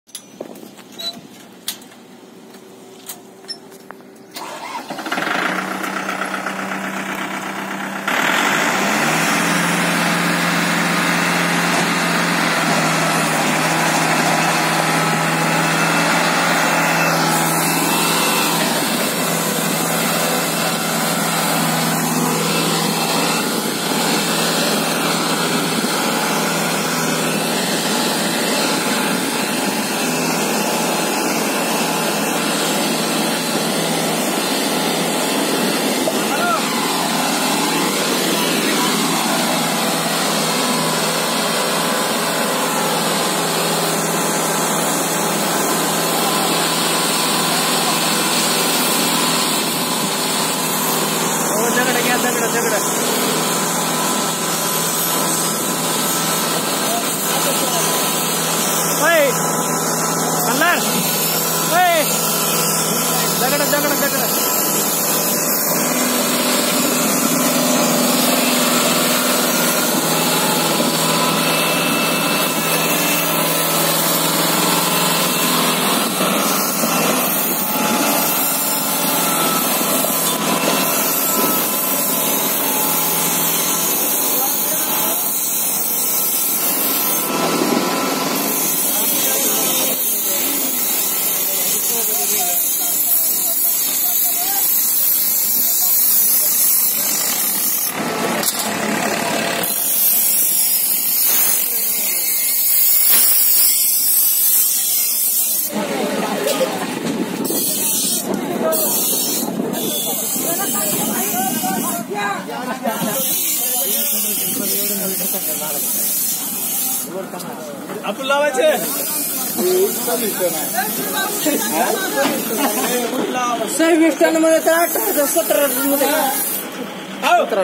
Mu5501 Kubota 55Hp Tractor Back Side Attack 2 Trailer In Sugarcane Hevi Lod
Best tractors made in by Japan.